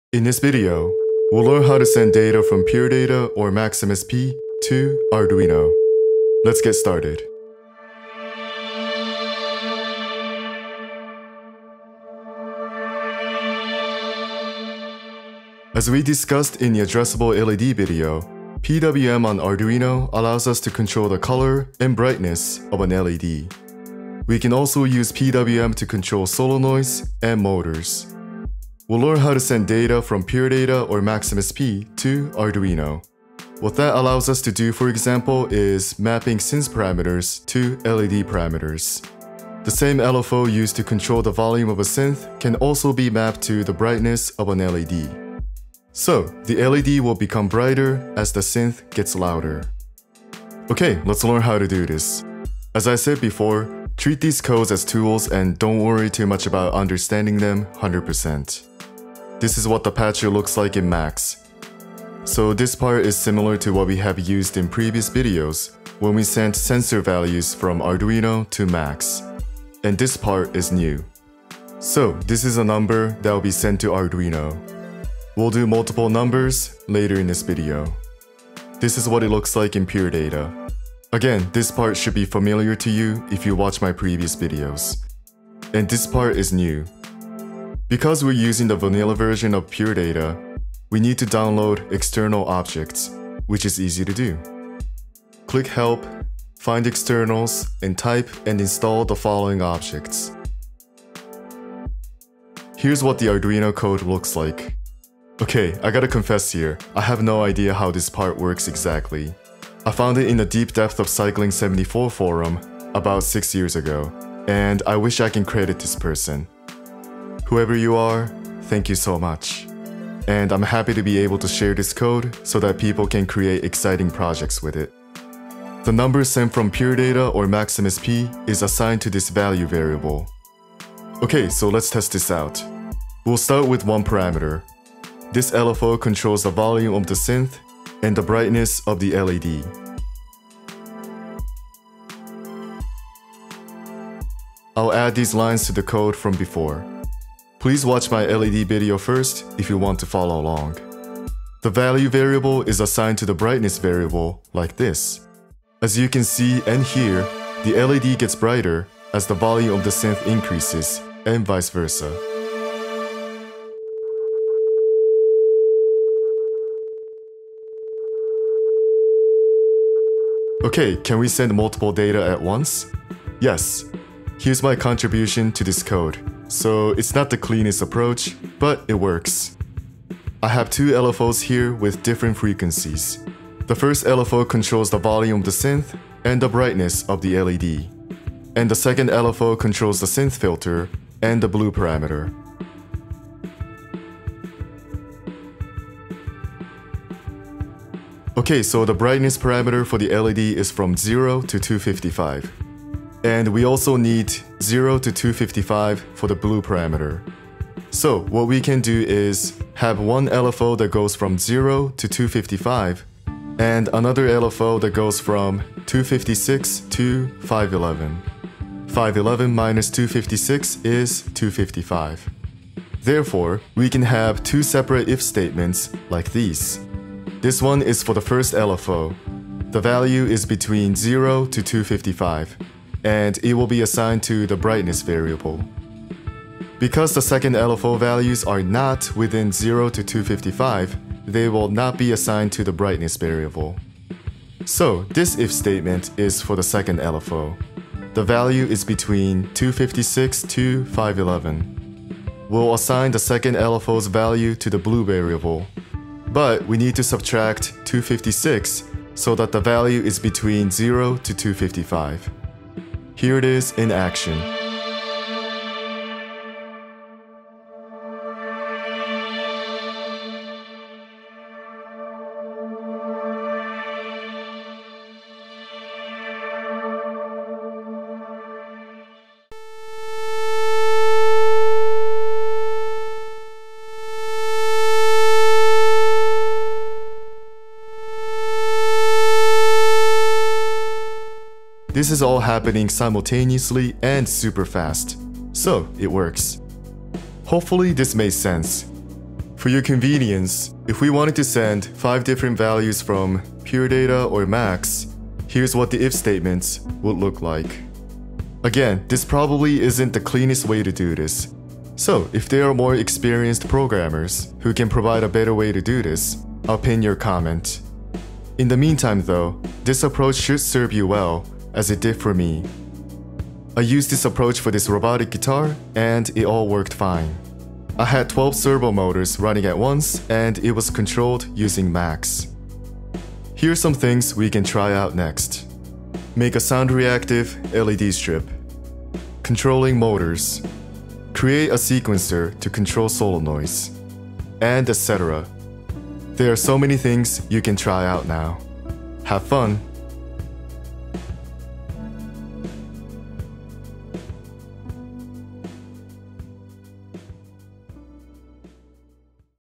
Max/MSP to Arduino | Pure Data to Arduino
Let’s learn how to send data from Max/MSP or Pure Data to Arduino! We’ll control an addressable LED strip using an LFO! And we'll learn how to send multiple numbers as well!!

0:00 - Intro
0:51 - Max & Pure Data patches
1:42 - Arduino code
2:11 - Sending one number
2:51 - Sending multiple numbers
4:52 - Outro